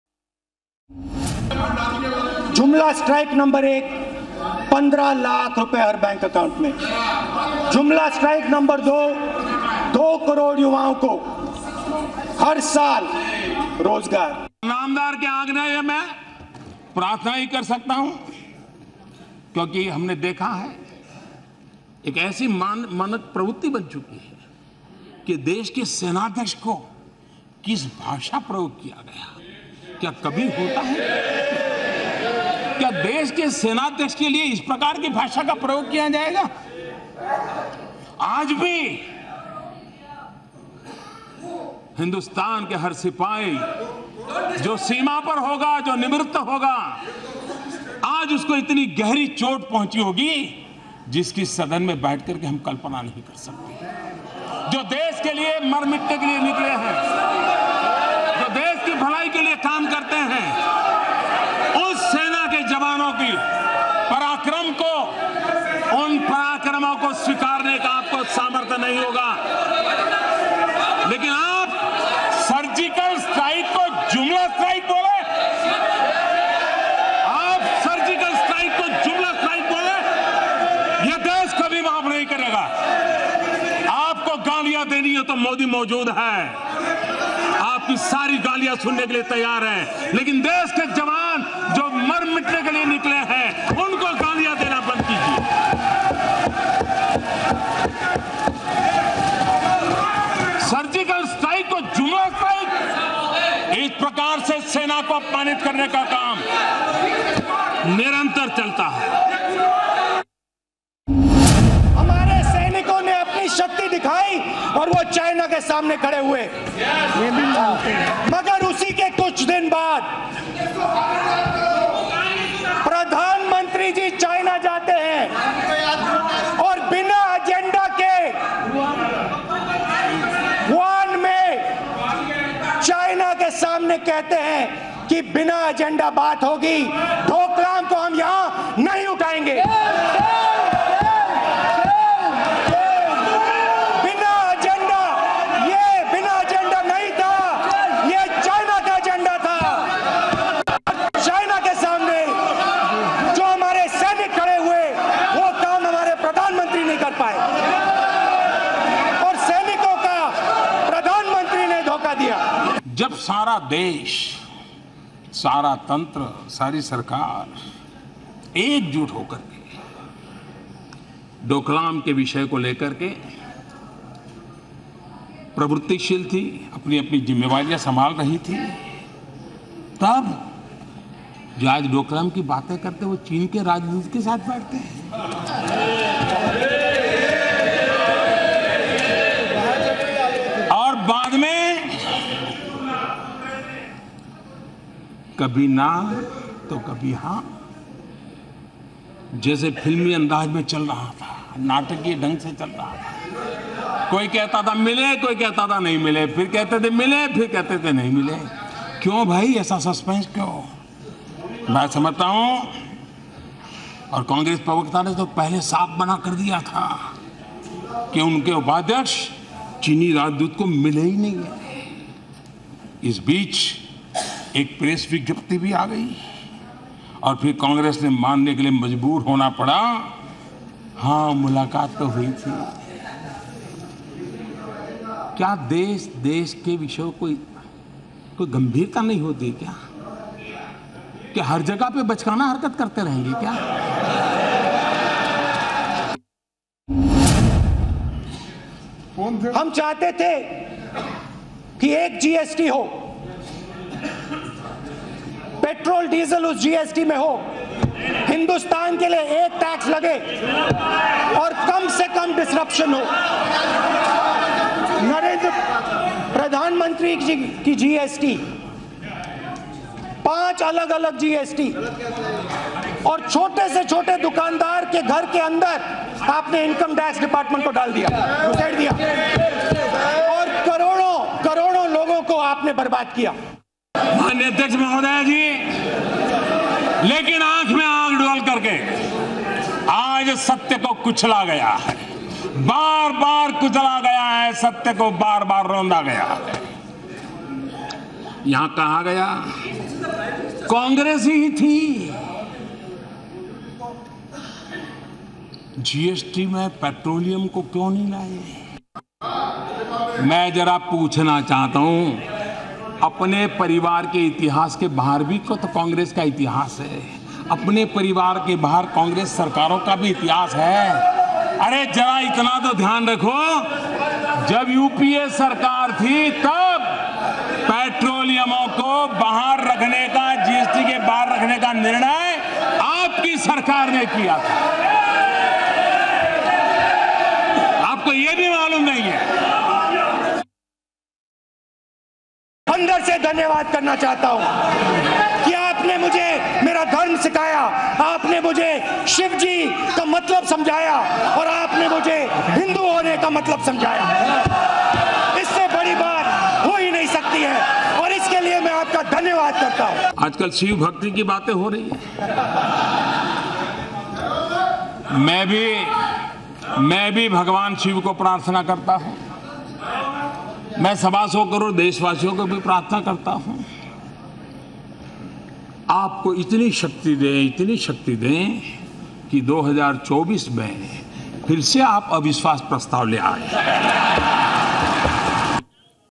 Rahul Vs Modi : Rahul Gandhi के सवालों पर PM Modi के चुटील जवाब
लोकसभा में अविश्साव प्रस्ताव पर जोरदार बहस हुई, Modi सरकार ने 325 मतों से विश्वास जीत लिया, जो पहले से तय था लेकिन सबकी नजरें टिकी थी Rahul Gandhi के वार और मोदी के पलटवार पर Rahul ने लोकसभा में चुन-चुनकर PM Modi ने हमले किया और फिर जब पीएम मोदी की बारी आई तो सब धुआं हो गया, राहुल की ऐसी बोलती बंद हुई की वो संसद भवन की छत देखने लगे...देखिए

About Channel